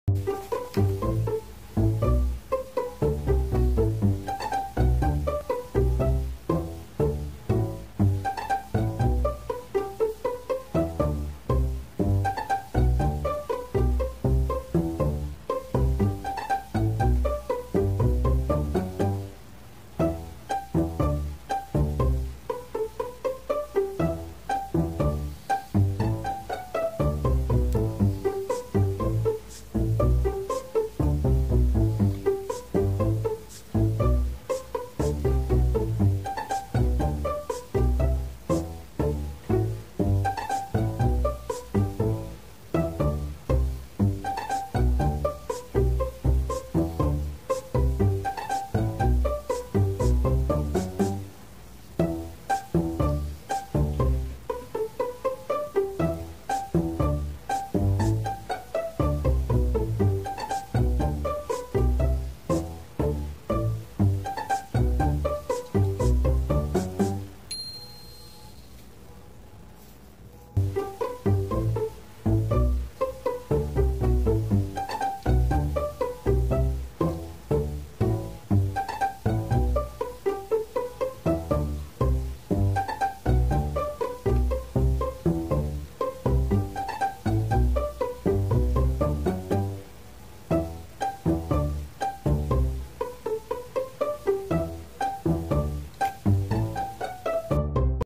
Cat with thumbs must test water before he drinks
Cat taps paws in water